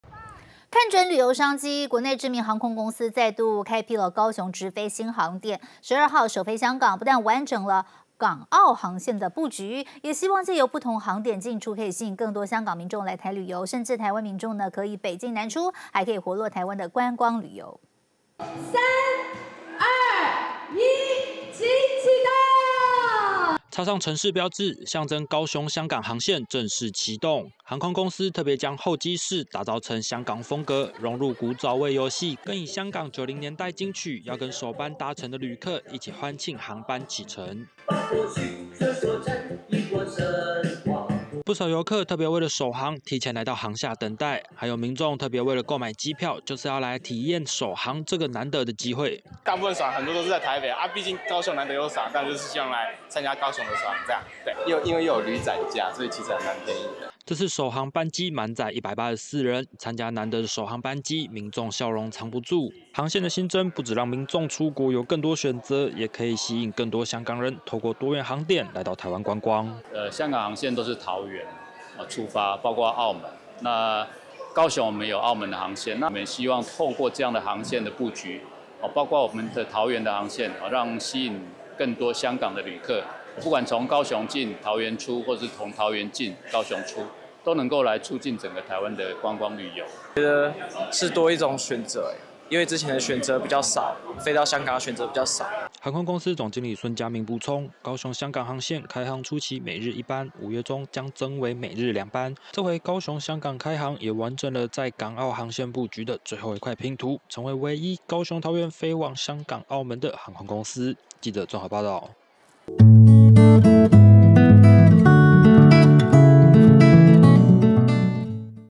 航空公司「高雄-香港」航線首航 班機旅客爆滿
看準旅遊商機，國內知名航空公司再度開闢「高雄直飛」新航點，12日首飛香港。不但完整了港澳航線布局，也希望藉由不同航點進出，吸引更多香港民眾來台旅遊，甚至台灣民眾可以「北進南出」，還能活絡台灣的觀光旅遊。

下載【年代電視】APP：
iOS平台：